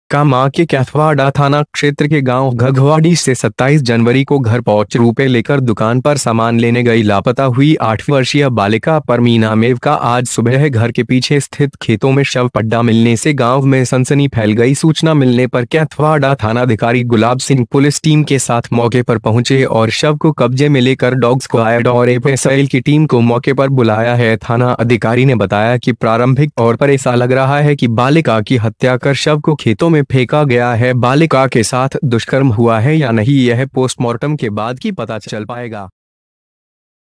8 साल की बच्ची का मिला घर के पीछे ही शव
कामां के कैथवाडा थाना क्षेत्र के गांव घघवाडी से 27 जनवरी को घर पॉच रूपये लेकर दुकान पर सामान लेने गई लापता हुई आठ वर्षीय बालिका परमीना मेव का आज सुबह घर के पीछे स्थित खेतों में शव पडा मिलने से गांव में सनसनी फैल गई सूचना मिलने पर कैथवाडा थाना धिकारी गुलाब सिंह पुलिस टीम के साथ मौके पर पहुंचे और शव को कब्जे में लेकर डॉग स्क्वायड और एफएसएल की टीम को मौके पर बुलाया है थाना अधिकारी ने बताया कि प्रारंभिक तौर पर एेसा लग रहा है कि बालिका की हत्या कर शव को खेतों में फेका गया है बालिका के साथ दुष्कर्म हुआ है या नही यह पोस्टमार्टम के बाद की पता चल पाएगा